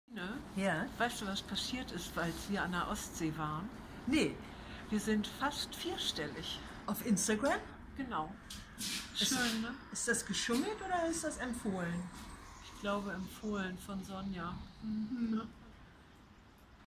DoFi 310 In eigener Sache
Lesezeit an der Ostsee: Vorbereitung des Einkaufs im Frühjahr und schwupps sind wir bei einer 4stelligen Zahl von instagram Followern!